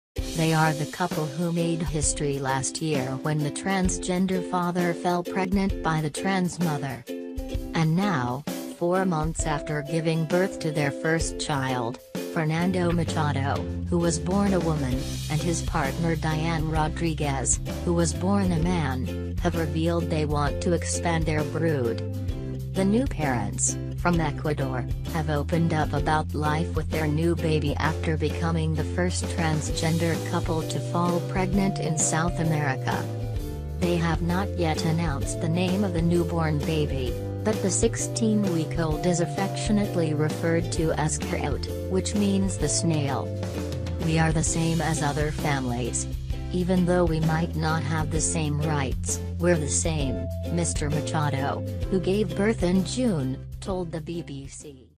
THIS IS THE END!!!!!
Ecuadorian transgender man Fernando Machado gives birth to baby